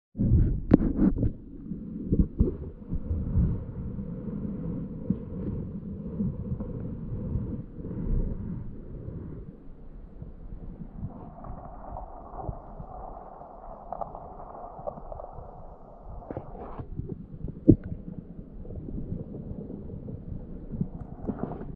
Crucian swimming underwater, latin: Carassius.
One friendly Crucian swimming through the sunny waters of Latvian lake.